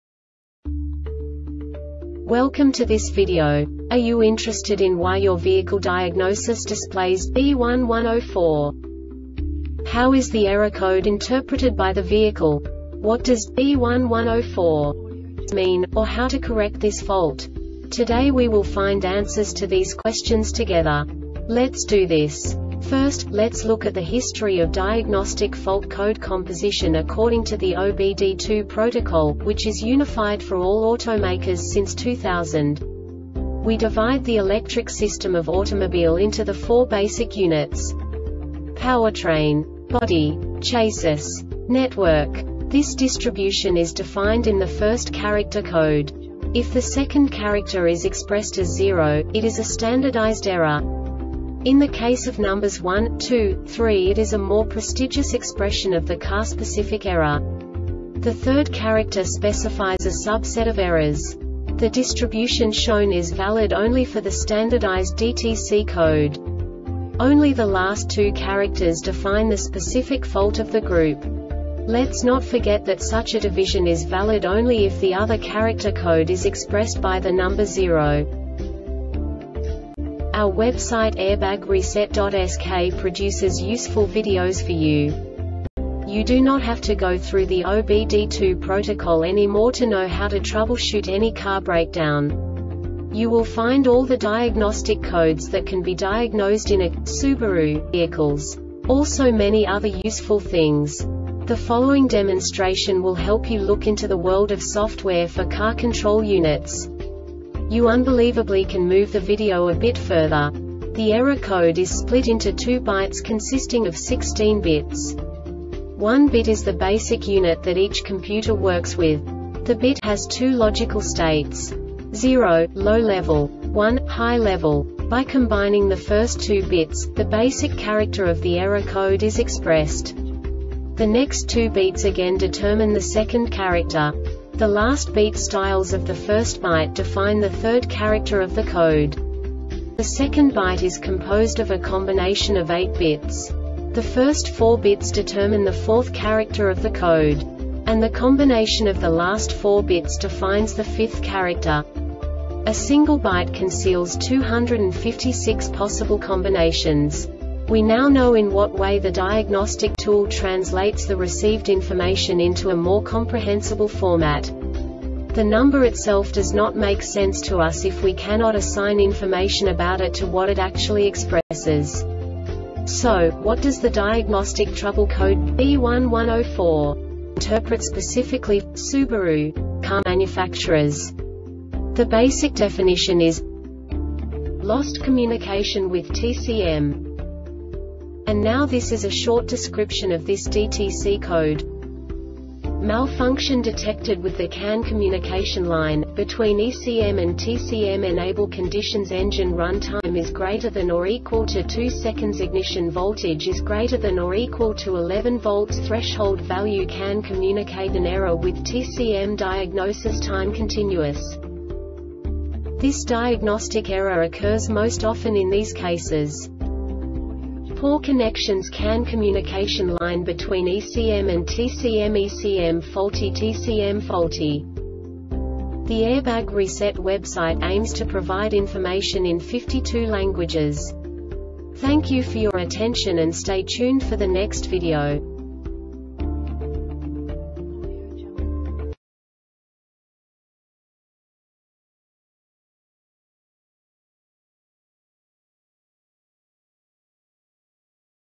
DTC Subaru B1104 Short Explanation
Contents:
0:21 Basic DTC analysis according to OBD2 protocol standard.
1:48 Insight into programming
2:58 A diagnostically understandable description of the Diagnostic Trouble Code
3:05 Basic error definition

Make:
Subaru
Code:
B1104
Definition:
Lost Communication With TCM
Description: